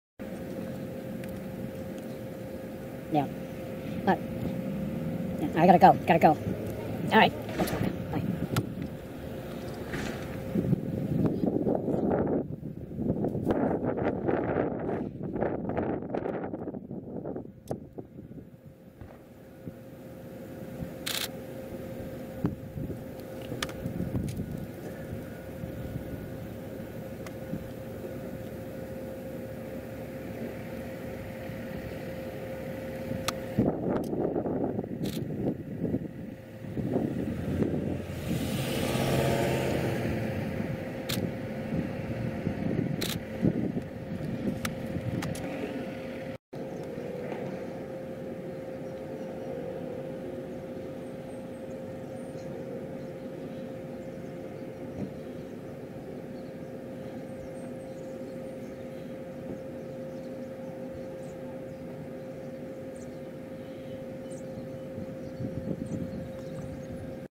PEREGRINES ON POLE, FEMALE WITH PREY, THURS. MARCH 16, 2023 1 4323 4324
PEREGRINES ON POLE, FEMALE WITH PREY, THURS. MARCH 16, 2023